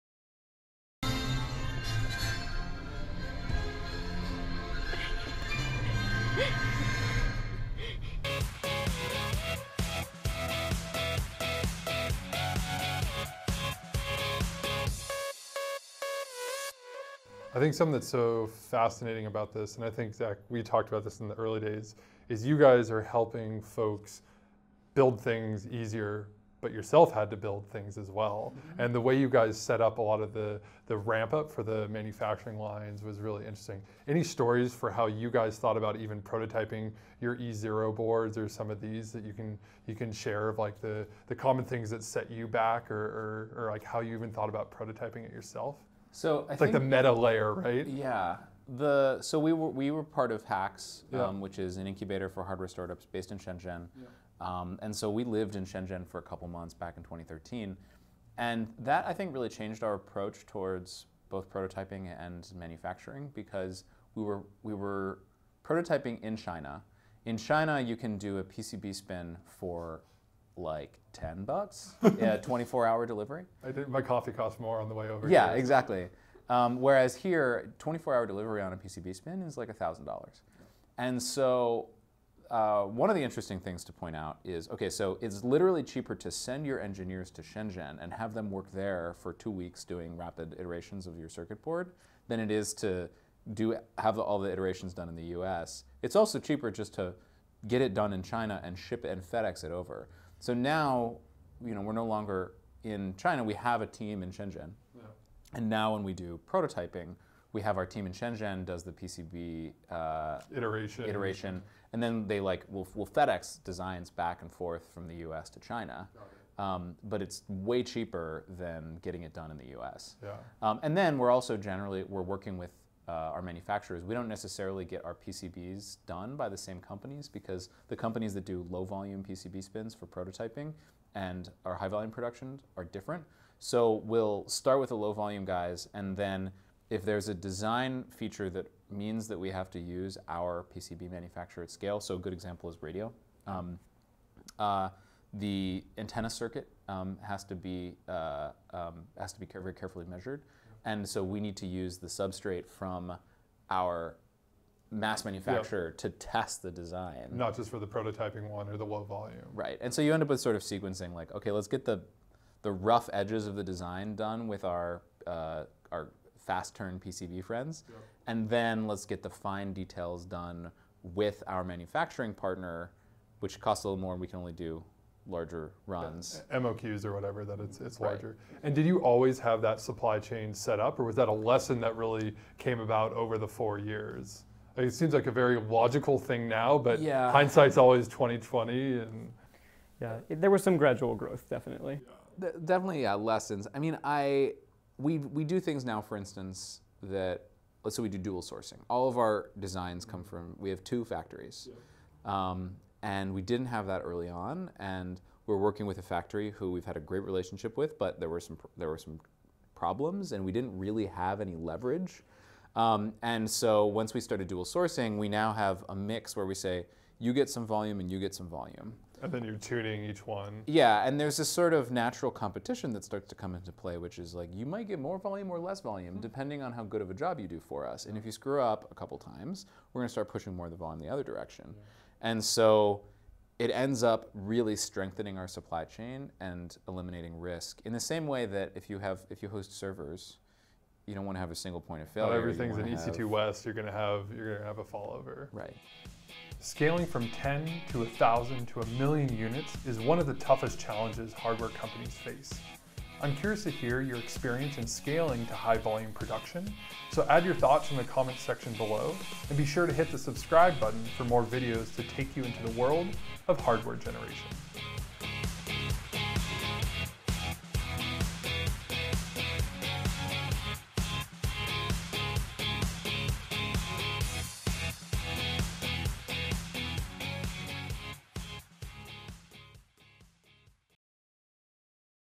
Particle Founders on How to Scale from 10 to 1M Units [PART 3] | Fictiv
In Part 3 of our IoT series with Particle, Dave chats with Particle Co-founders Zach Supalla and Zachary Crockett about best practices for scaling from 10 to 1M units manufactured.